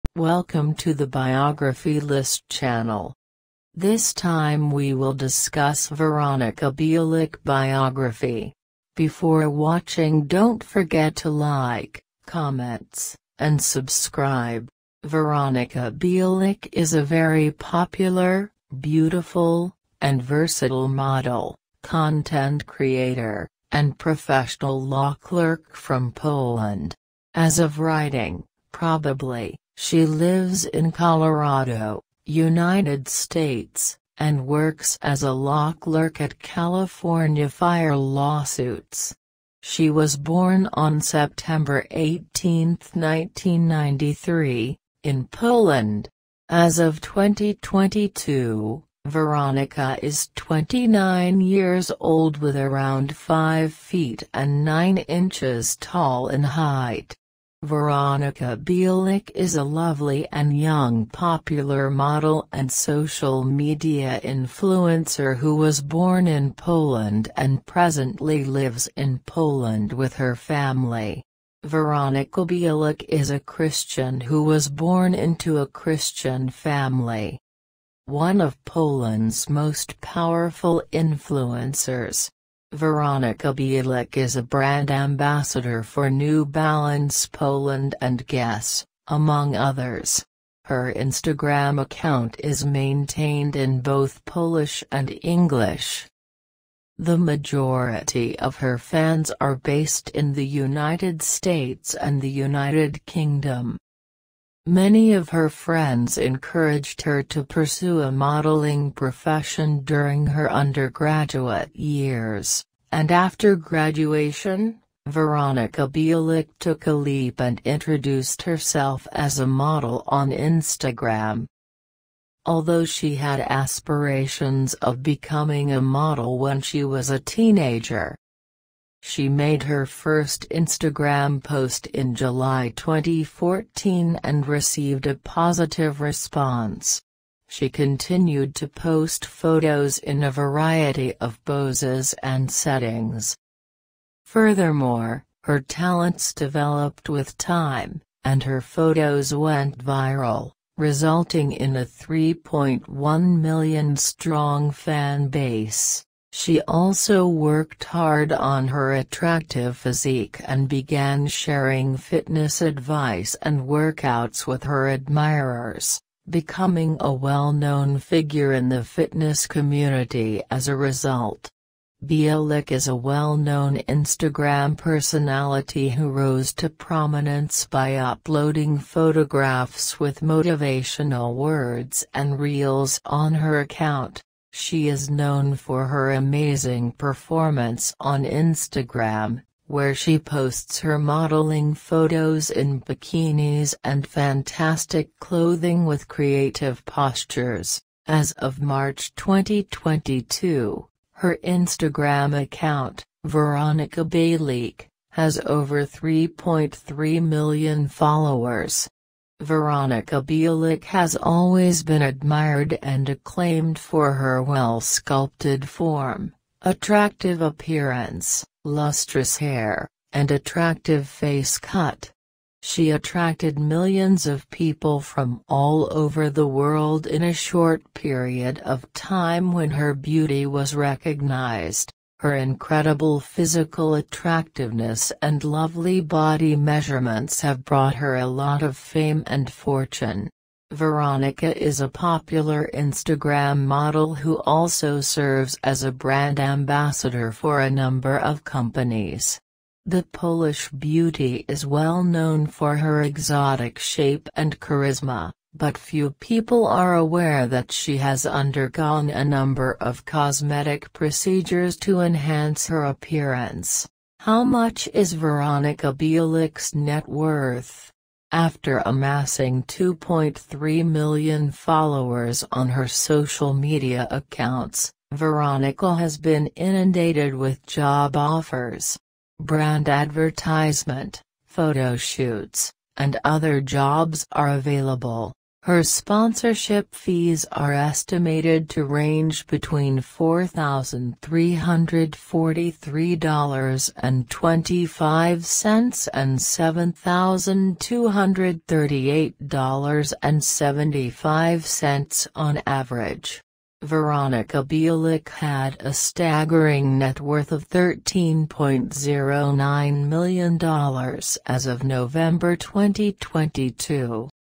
Model Poland Veronica Bielik Biography : Life, Measurements, Age, Christian Harding, Net Worth, wiki
Veronica Bielik is a very popular, beautiful, and versatile Model, content creator, and professional Law clerk from Poland. As of writing, probably, she lives in Colorado, United States, and works as a Law Clerk at California Fire Lawsuits. She was born on September 18, 1993, in Poland. As of 2022, Veronica is 29 years old with around 5 feet and 9 inches tall in height.

Highlights: Biography, Biodata, Weight, Height, Career, Images, Videos & much more...